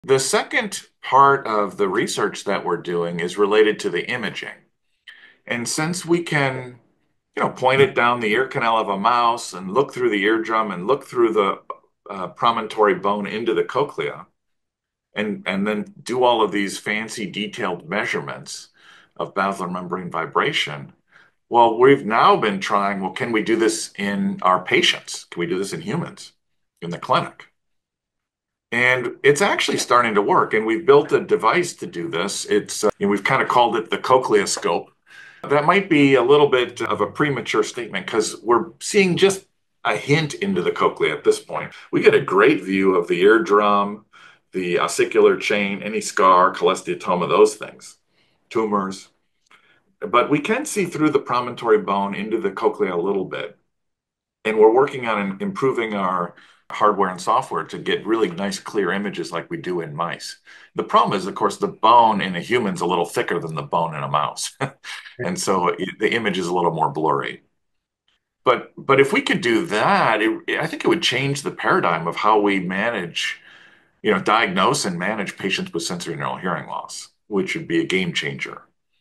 How OCT Imaging Could Transform Hearing Loss Diagnosis and Treatment
What if a cutting-edge imaging technology could revolutionize how we diagnose and treat hearing loss? Dr. John Oghalai discusses the exciting potential of Optical Coherence Tomography (OCT), which could allow clinicians to gain deeper insights into cochlear health and sensorineural hearing loss. Dr. Oghalai explains how this innovative technology, once refined, could revolutionize the diagnosis and treatment of hearing loss in the ENT and audiology clinic.